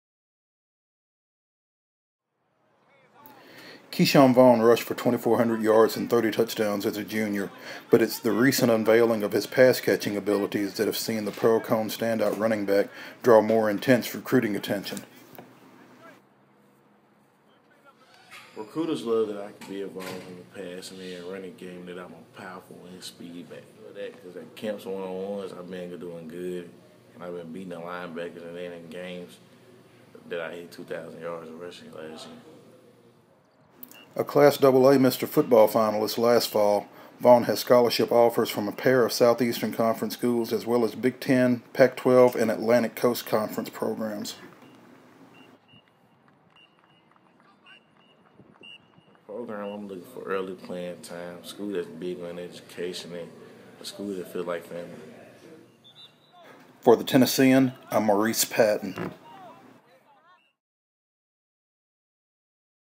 Pearl-Cohn running back on the rise
Pearl-Cohn's KeShawn Vaughn is receiving increased recruiting attention as his senior year approaches